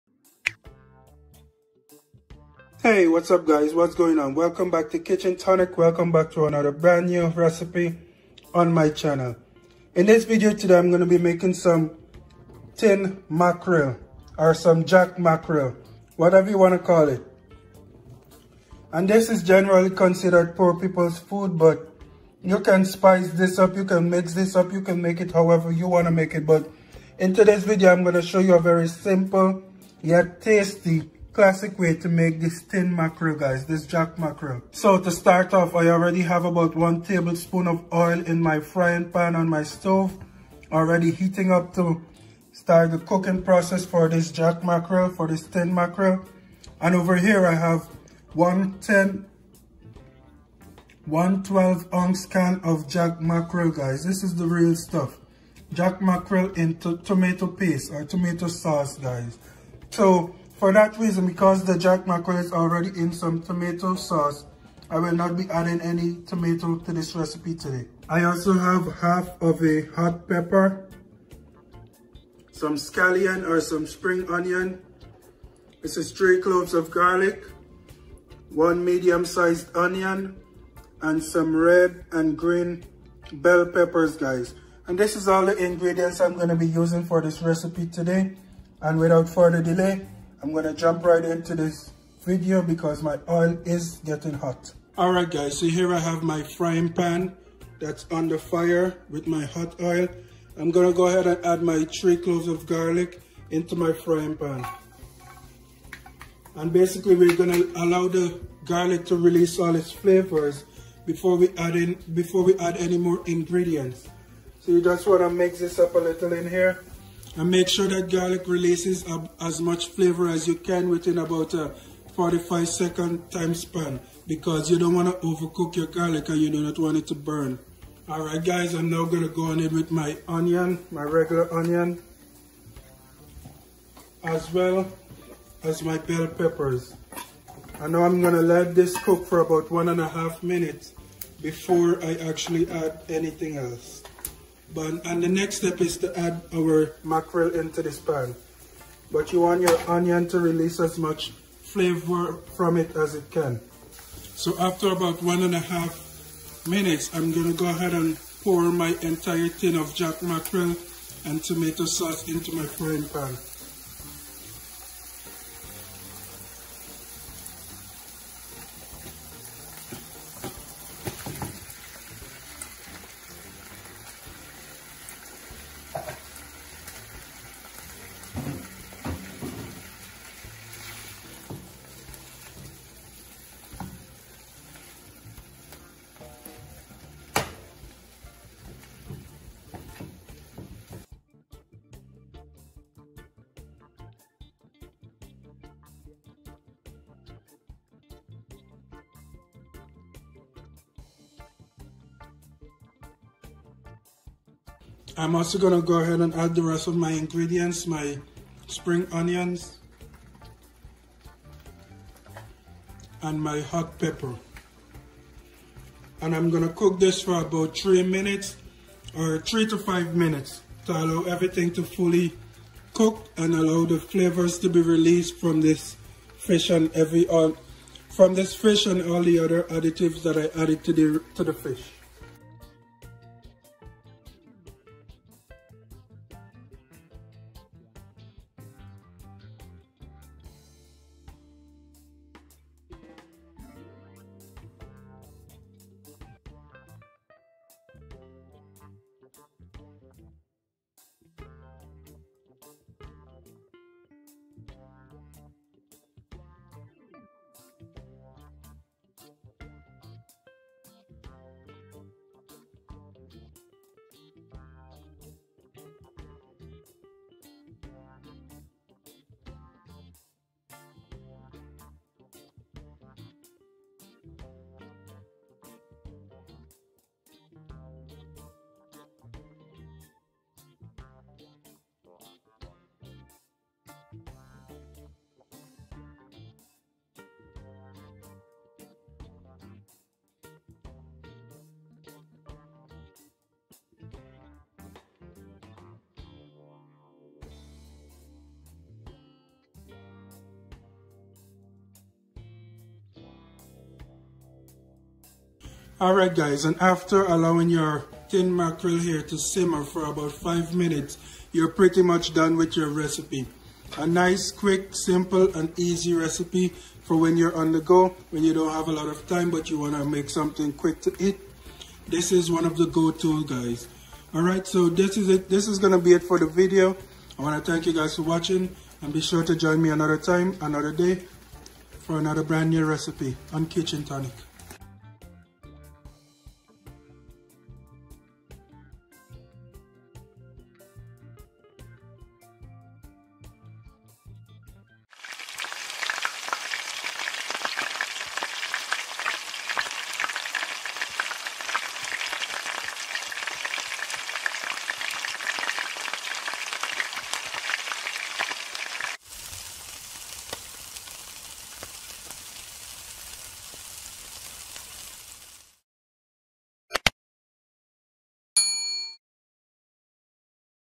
How To Cook Jack Mackerel | Jamaican Tin Mackerel Recipe
Ingredients Used In Video
(1) 12 Oz Can Of Jack Mackerel
½ Hot Pepper
Scallion
3 Cloves Garlic
1 Medium Sized Onion
½ Green Bell Pepper
½ Red Bell Pepper